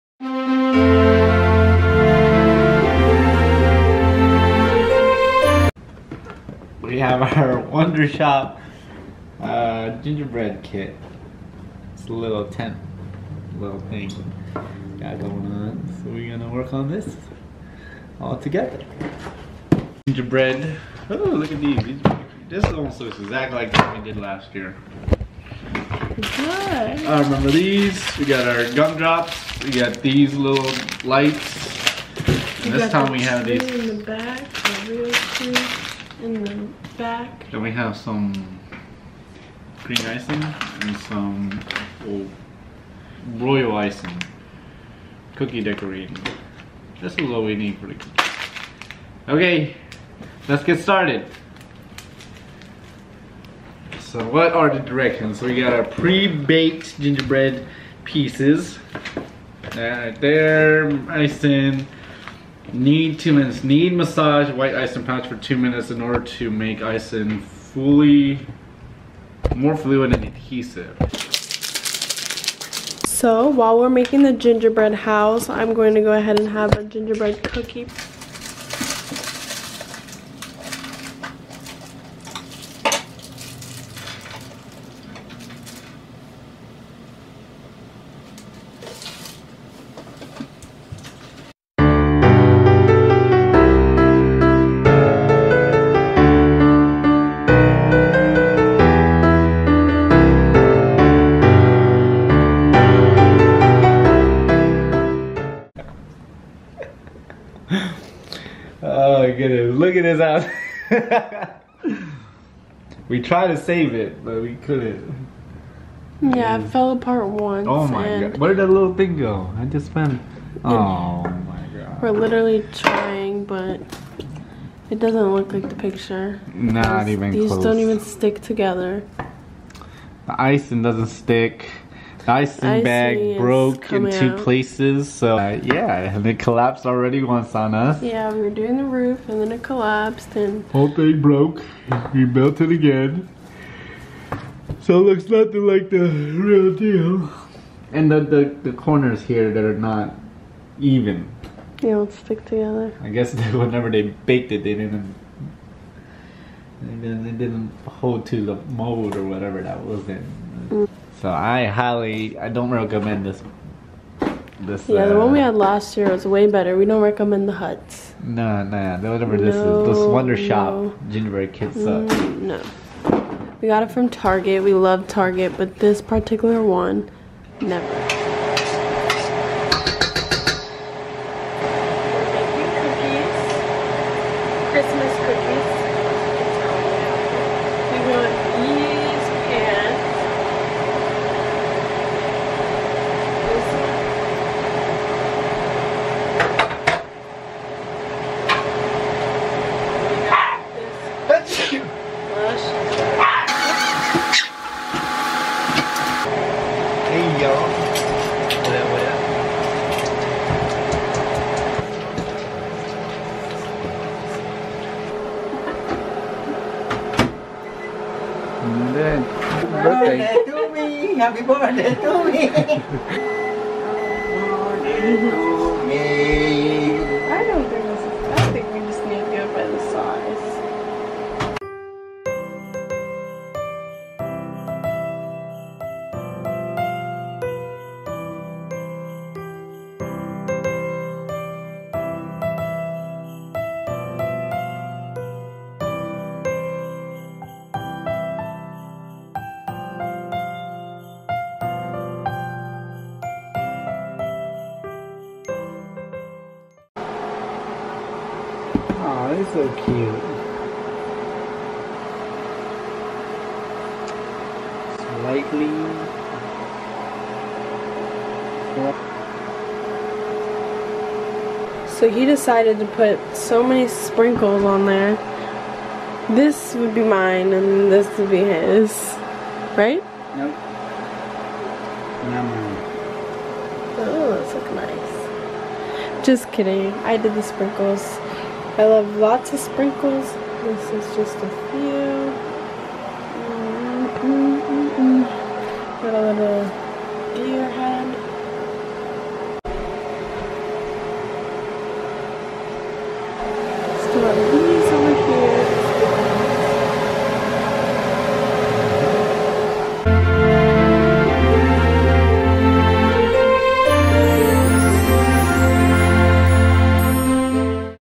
Making our Gingerbread house and Christmas Cookies!!
Join my boyfriend in I as we make our 2019 gingerbread house and Christmas cookies!!